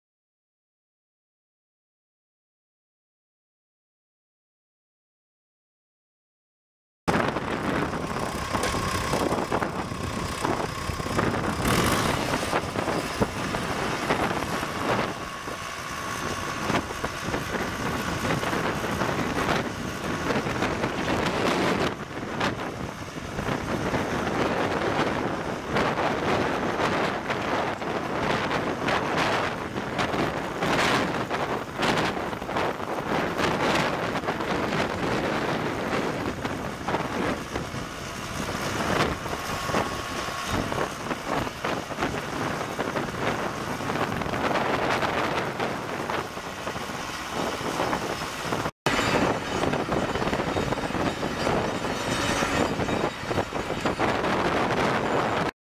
1988 Versatile 276 Bi-directional 4WD tractor for sale | sold at auction May 8, 2013
SOLD! H and T Farms Retirement Auction

1988 Versatile 276 Bi-directional 4WD tractor
5,452 hours on meter
Cummins 4BTA-3.9 four cylinder diesel engine
116 HP
Serial 44222212
Hydrostatic transmission
3F-3R gears
AC and heat
AM/FM/cassette
Spring seat
Versatile 2360 loader
8&#039; bucket
Quick attach
Hydraulic grapple for front loader bucket
Three point
Arms unattached
Serial 334331
Draw bar
540/1000 PTO
(3) rear hydraulic remote sets
39 gallon fuel tank
86" wheelbase
16.9-28 tires
Operators manual
Cracked window
Bale monitor will be removed
Serial 332346

It was located in Shattuck, OK, and we sold it to the highest bidder for $17,050.